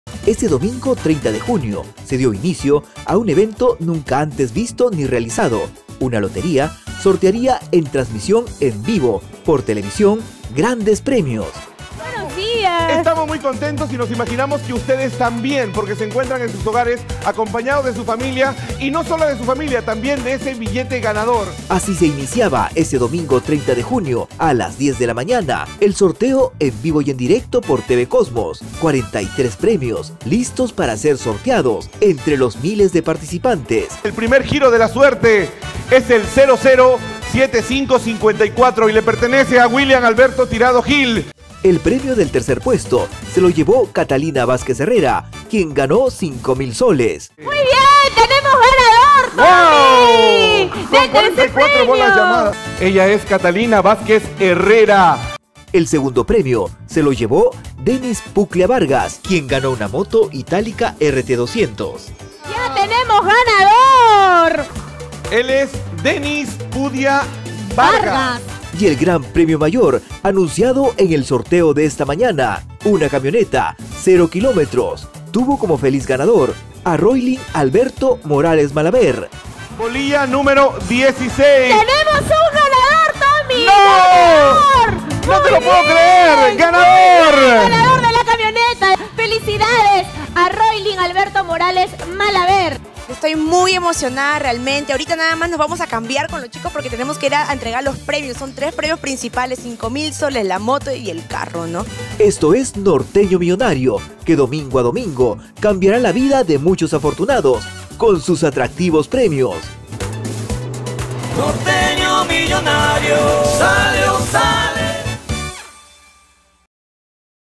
Los primeros ganadores de "Norteño Millonario"
Este 30 de junio se realizó el sorteo de Norteño Millonario.

El domingo 30 de junio se inició a las 10 de la mañana el sorteo en vivo y en directo por TV Cosmos, 43 premios listos para ser sorteados entre los miles de participantes.